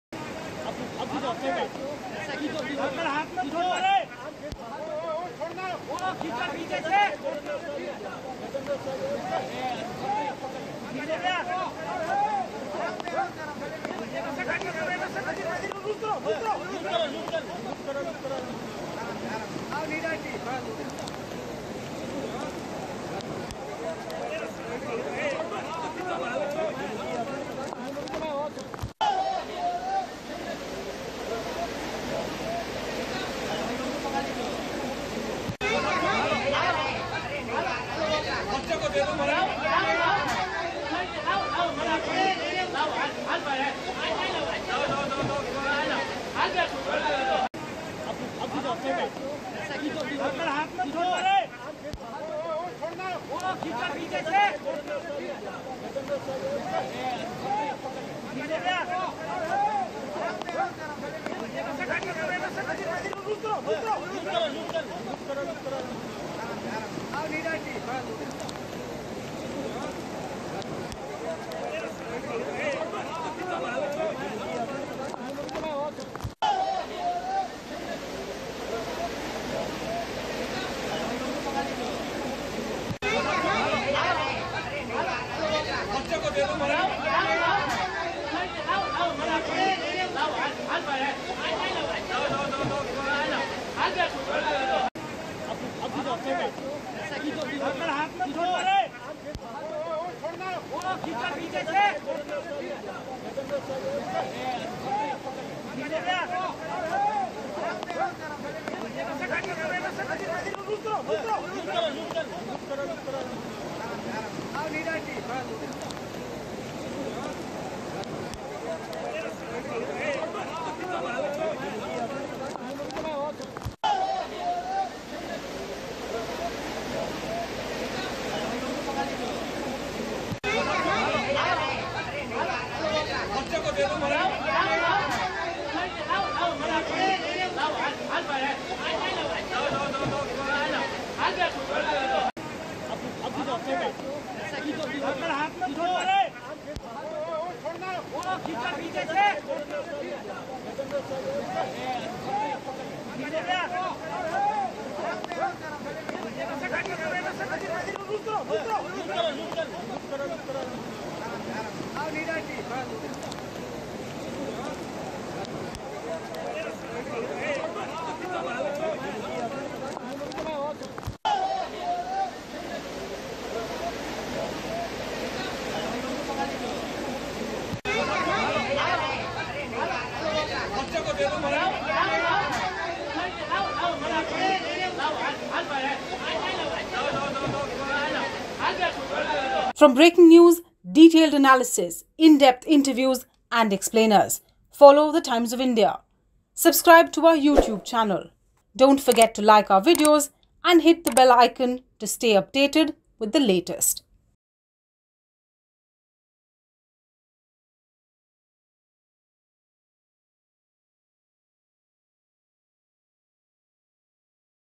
Heavy rains cause flooding in low-lying areas in Junagadh, NDRF rescues several people
Heavy rains in Junagadh led to flood-like situation in low-lying areas of the district. The NDRF rescued several people who were stranded due to rising water level. Many areas in Gujarat are facing threat of floods due to heavy rains in the past 24 hours. Red alert has been issued for Junagadh due to incessant rains.